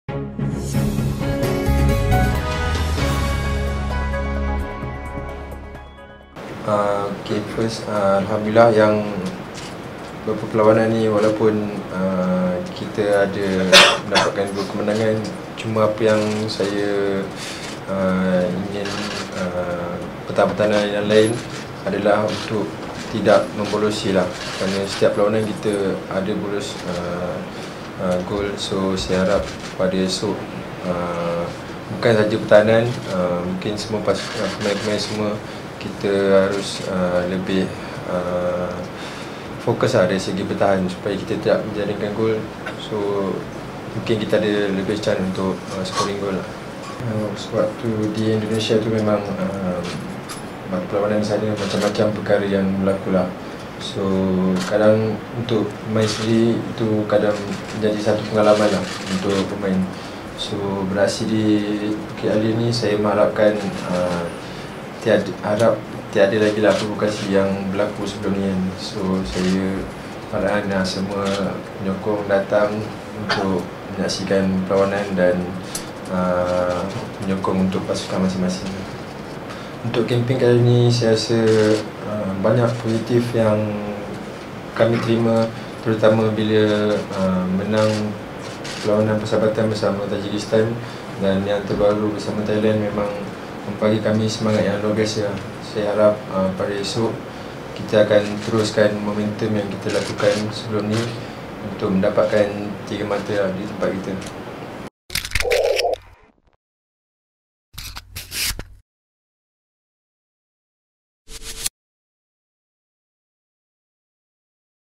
SUKAN MG 🎥 | Penyokong Harimau Malaya "Jangan Provokasi Penyokong Indonesia" - Shahrul Saad
KUALA LUMPUR – Peminat bola sepak negara ini diharap tidak mengulang insiden yang dilakukan oleh penyokong Indonesia kepada pemain dan penyokong Harimau Malaya di Stadium Gelora Bung Karno di Jakarta pada September lalu.

Sebaliknya bek tengah negara, Shahrul Saad berkata, peminat negara harus mengamalkan semangat kesukanan yang tinggi pada aksi timbal balik menentang negara republik itu di Stadium Nasional Bukit Jalil esok.

Jelas pemain Perak itu, Malaysia perlu menghormati tetamu biarpun pernah dipalit episod hitam di tempat lawan.

“Macam-macam insiden yang berlaku di Jakarta tempoh hari dan telah memberi pengalaman kepada pemain.